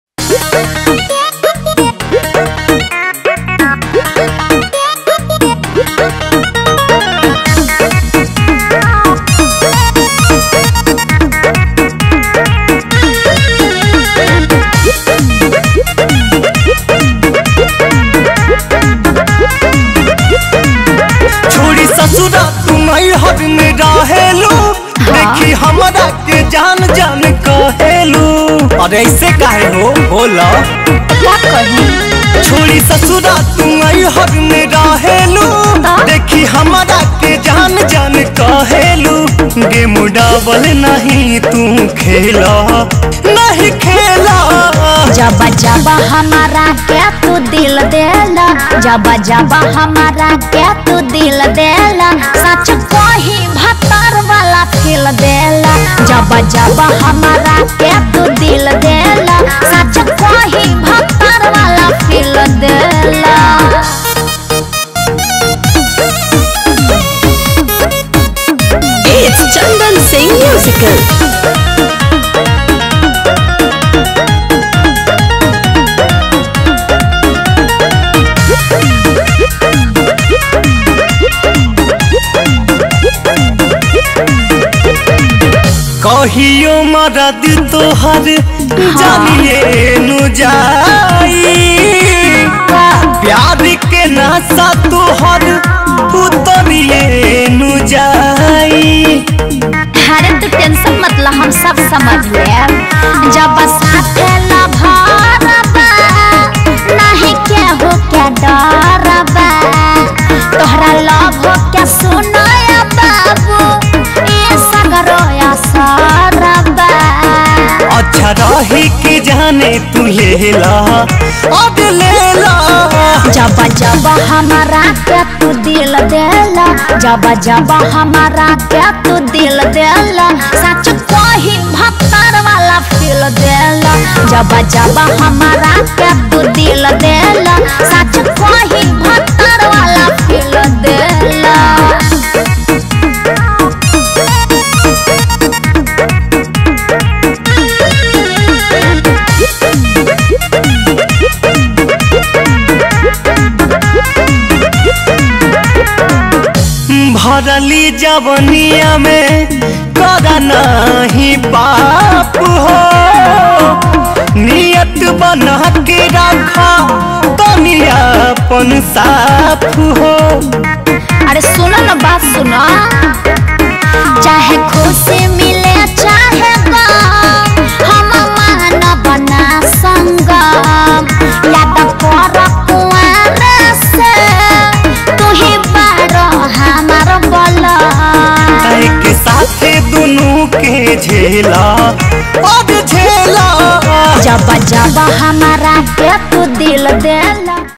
bhojpuri_song || Kajal || डांसर काजल का धमाकेदार डांस विडियो भोजपुरी सोंग 2023
Singer : Rahul Jadeja & Antra Singh Priyanka

Writer : Shailesh Sangam

Music : Chandan Singh

Actor : Kajal